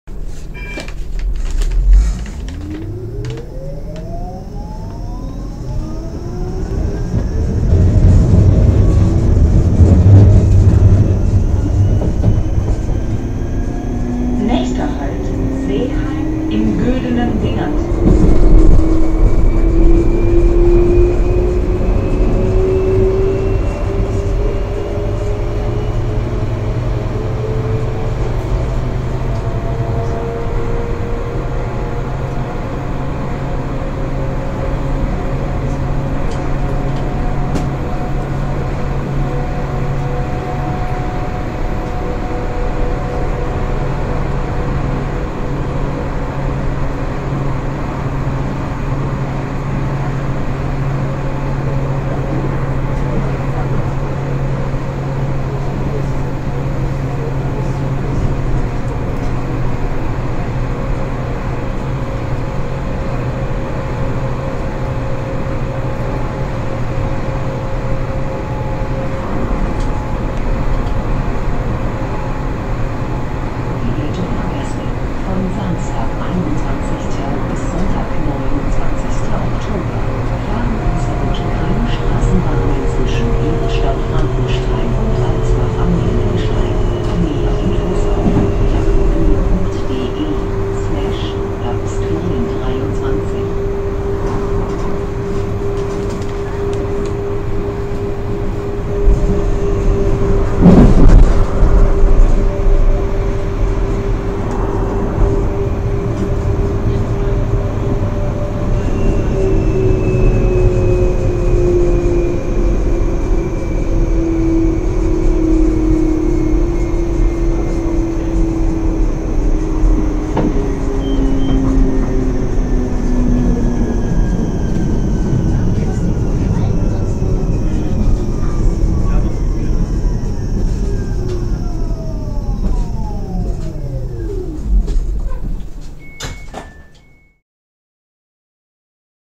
Darmstadt ST15 [Tina] Sound 70km/h
Heute am 09.10.23 war ein wichtiger Tag .
Nach langem warten war einer der neuen ST15 TW22101 das erste mal mit Fahrgästen auf Linie unterwegs.

Aufgenommen zwischen Malchen - Seeheim.